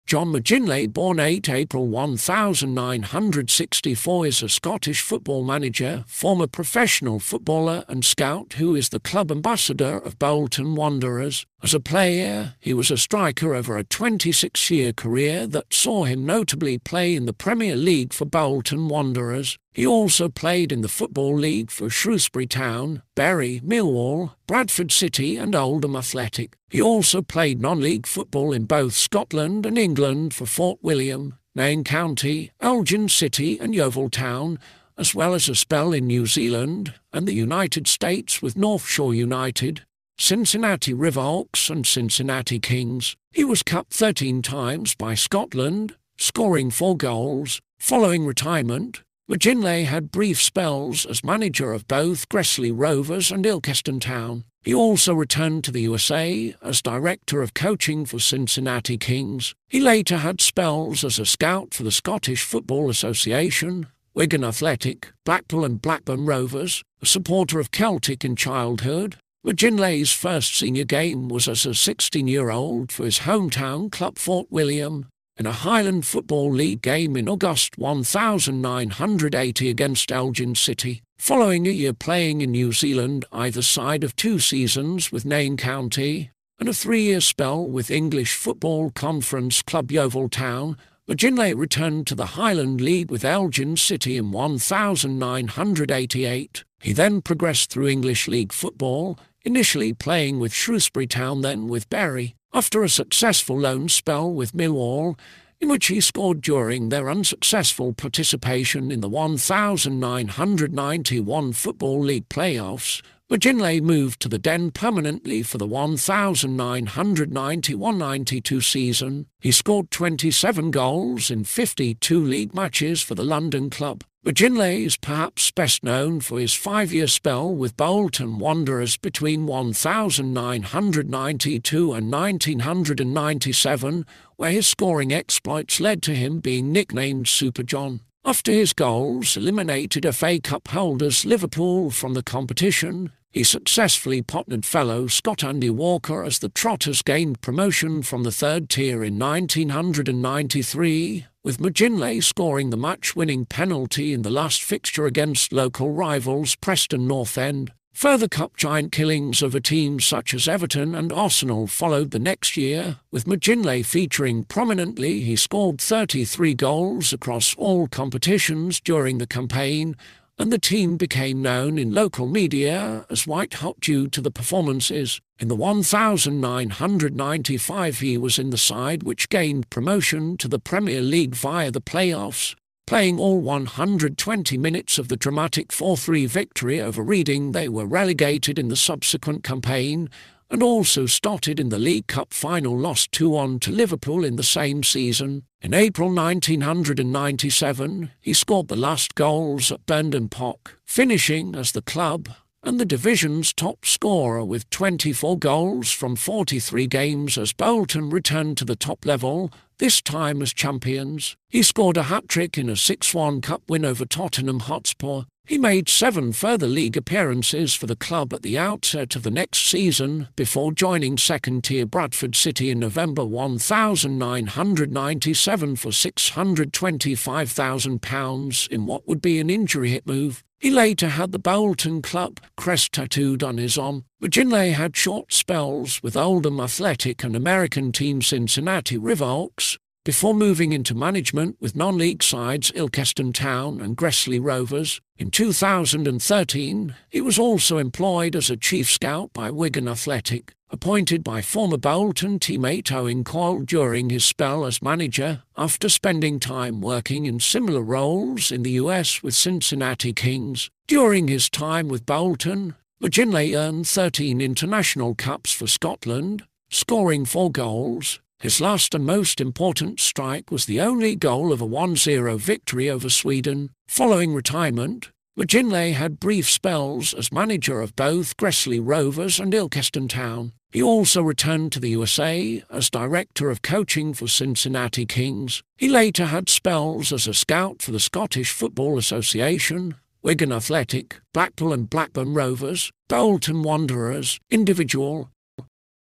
John McGinlay Biography
This video is about John McGinlay Biography in English.
John McGinlay (born 8 April 1964) is a Scottish football manager, former professional footballer and scout who is the club ambassador of Bolton Wanderers.
As a player, he was a striker over a 26-year career that saw him notably play in the Premier League for Bolton Wanderers. He also played in the Football League for Shrewsbury Town, Bury, Millwall, Bradford City and Oldham Athletic. He also played non-league football in both Scotland and England for Fort William, Nairn County, Elgin City and Yeovil Town, as well as a spell in New Zealand and the United States with North Shore United, Cincinnati Riverhawks and Cincinnati Kings. He was capped 13 times by Scotland, scoring four goals.
Following retirement, McGinlay had brief spells as manager of both Gresley Rovers and Ilkeston Town. He also returned to the USA as director of coaching for Cincinnati Kings. He later had spells as a scout for the Scottish Football Association, Wigan Athletic, Blackpool and Blackburn Rovers.
A supporter of Celtic in childhood, McGinlay's first senior game was as a 16-year-old for his hometown club Fort William, in a Highland Football League game in August 1980 against Elgin City. Following a year playing in New Zealand either side of two seasons with Nairn County, and a three-year spell with English Football Conference club Yeovil Town, McGinlay returned to the Highland League with Elgin City in 1988.
He then progressed through English League football, initially playing with Shrewsbury Town then with Bury. After a successful loan spell with Millwall, in which he scored during their unsuccessful participation in the 1991 Football League play-offs, McGinlay moved to The Den permanently for the 1991–92 season. He scored 27 goals in 52 league matches for the London club.
McGinlay is perhaps best known for his five-year spell with Bolton Wanderers between 1992 and 1997, where his scoring exploits led to him being nicknamed 'Super John'. After his goals eliminated FA Cup holders Liverpool from the competition, he successfully partnered fellow Scot Andy Walker as the Trotters gained promotion from the third tier in 1993, with McGinlay scoring the match winning penalty in the last fixture against local rivals Preston North End.
Further cup 'giant killings' over teams such as Everton and Arsenal followed the next year, with McGinlay featuring prominently – he scored 33 goals across all competitions during the campaign, and the team became known in local media as 'white hot' due to the performances. In the 1995 he was in the side which gained promotion to the Premier League via the play-offs, playing all 120 minutes of the dramatic 4–3 victory over Reading (they were relegated in the subsequent campaign), and also started in the League Cup Final (lost 2–1 to Liverpool) in the same season.
In April 1997, he scored the last goals at Burnden Park, finishing as the club and the division's top scorer with 24 goals from 43 games as Bolton returned to the top level, this time as champions. He scored a hat-trick in a 6–1 cup win over Tottenham Hotspur. He made seven further league appearances for the club at the outset of the next season before joining second-tier Bradford City in November 1997 for £625,000 in what would be an injury-hit move. He later had the Bolton club crest tattooed on his arm.
McGinlay had short spells with Oldham Athletic and American team Cincinnati Riverhawks before moving into management with non-league sides Ilkeston Town and Gresley Rovers. In 2013, he was also employed as a chief scout by Wigan Athletic,
appointed by former Bolton teammate Owen Coyle during his spell as manager, after spending time working in similar roles in the US with Cincinnati Kings (
During his time with Bolton, McGinlay earned 13 international caps for Scotland, scoring four goals; his last and most important strike was the only goal of a 1–0 victory over Sweden (
Bolton Wanderers
Individual